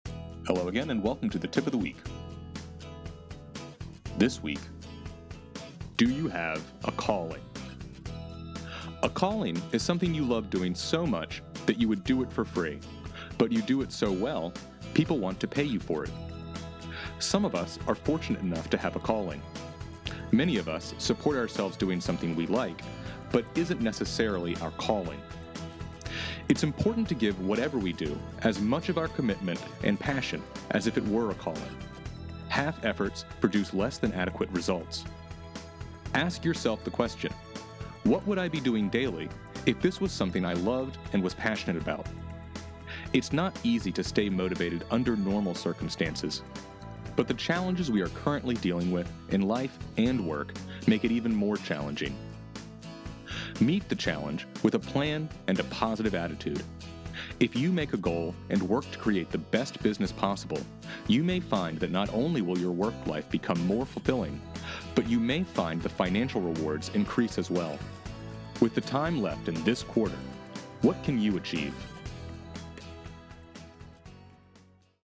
Do You Have a Calling?
A calling is something you love doing so much that you would do it for free, but you do so well, people want to pay you for doing it.
Some of us are fortunate enough to have a calling, many of us support ourselves doing something we like but isn’t necessarily our calling.  It is important to give whatever we do as much of our commitment and passion as if it were a calling.  Half efforts produce less than adequate results.  Ask yourself the question, “What would I be doing daily if this was something I loved and was passionate about?” It is not easy to stay motivated under normal circumstances, but the challenges we are currently dealing with in life and work make it even more challenging.  Meet the challenge with a plan and a positive attitude.  If you make a goal and work to create the best business possible, you may find that not only will your work life become more fulfilling, but you may find the financial rewards increase as well.  With the time left in this quarter what can you achieve?